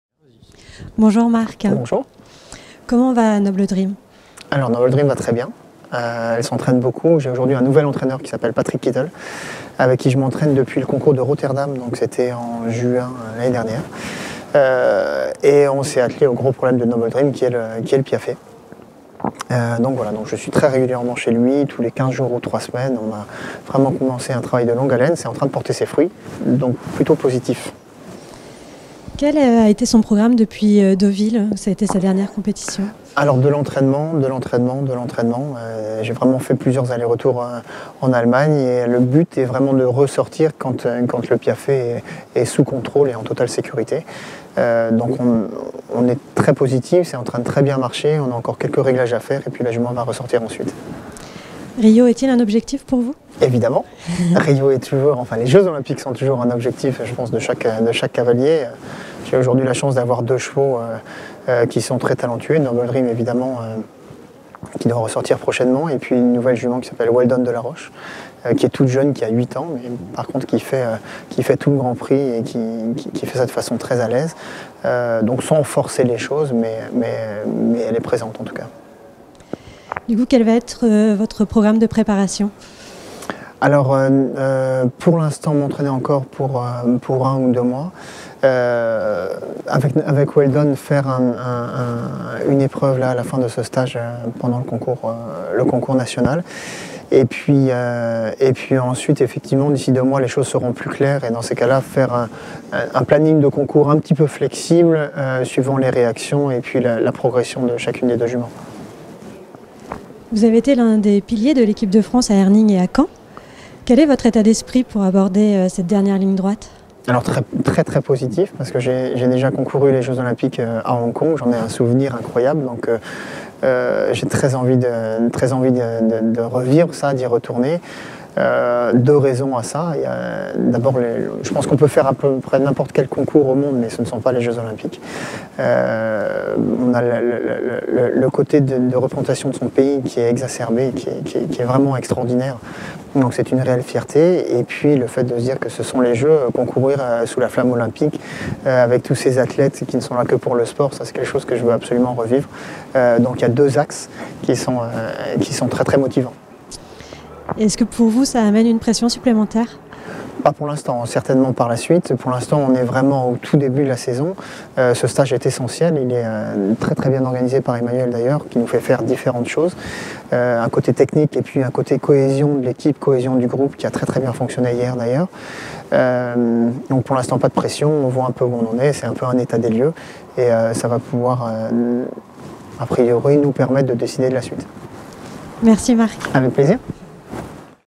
Interview Marc Boblet Le mans 2016
Interview du cavalier Marc Boblet lors du stage de préparation aux jeux olympiques de Rio 2016 qui se déroulait au Pole Européen du Cheval du Mans.